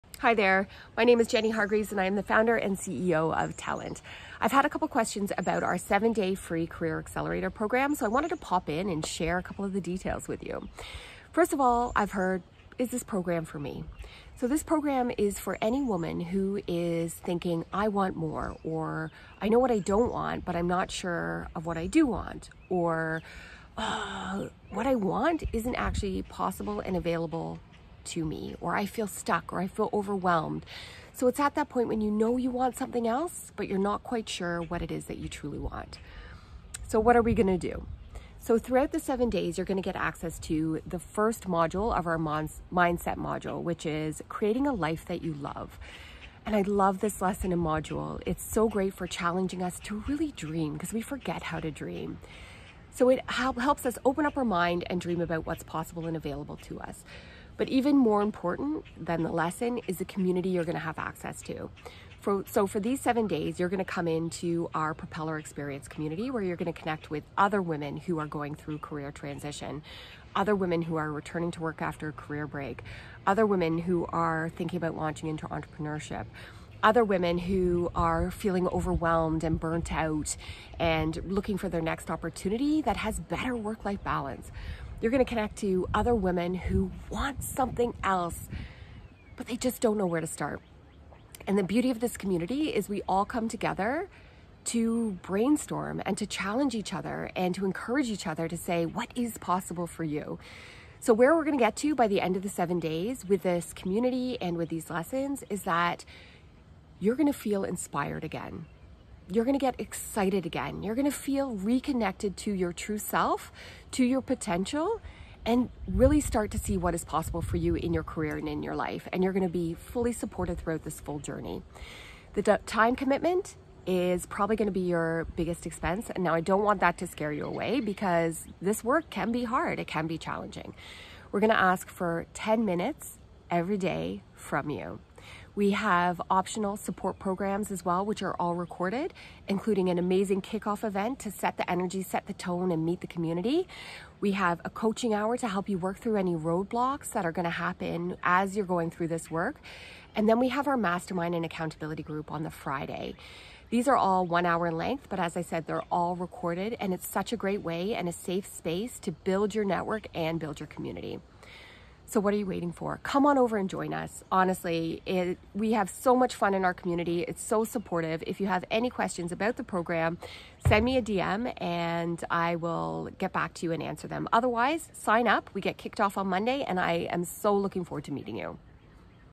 FREE 7 Day Challenge Career Accelerator
Reconnect with your potential. Reimagine your career.
Join us for this seven day Career Accelerator challenge and get clear on what it is that you truly want at work and in your life.

Your life and your talent is too precious to be wasting one more minute feeling overwhelmed, unfulfilled or stuck.

In seven days together, you will begin to adopt the mindset you need to not just see your potential, but get excited about it!

And you won’t do it alone. In addition to our guided digital program, you’re going to have access to a network of like minded women committed to doing the same work, coaches who will help you achieve breakthroughs, and alumni who can show you what is possible and support you to reimagine your goals and kick start your next career move.

It all starts Monday June 6th 2022!